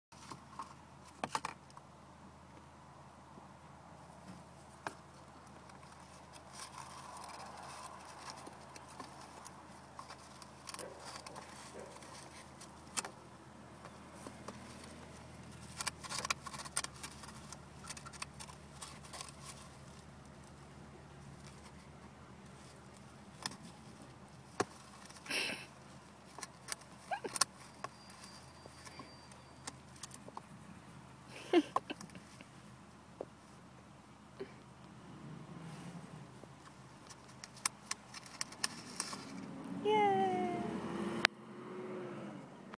Hedgehog playing with toilet paper tube
Our pet hedgehog, Sonic, playing/getting stuck in a toilet paper tube